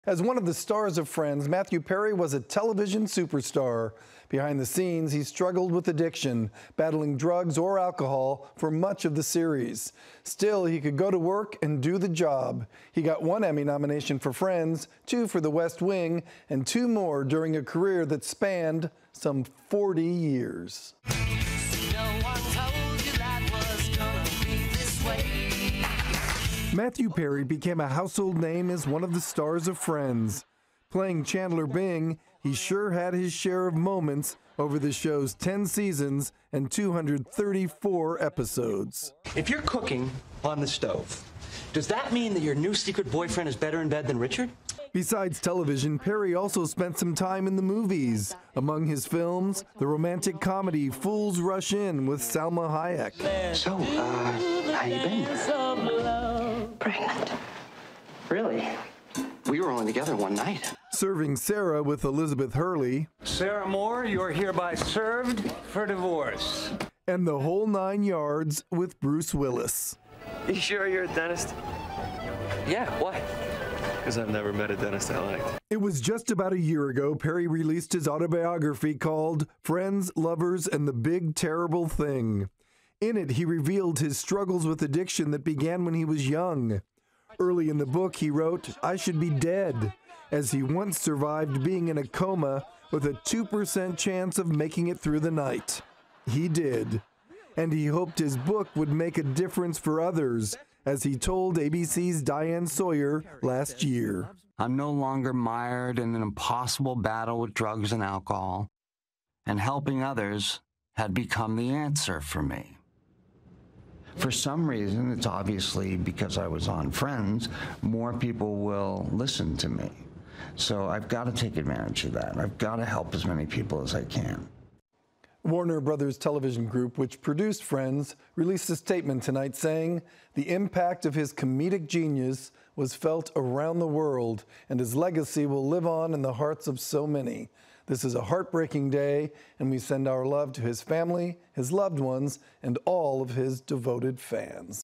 Remembering Matthew Perry
Matthew Perry was found unresponsive at his home in the Pacific Palisades around 4 p.m. Saturday.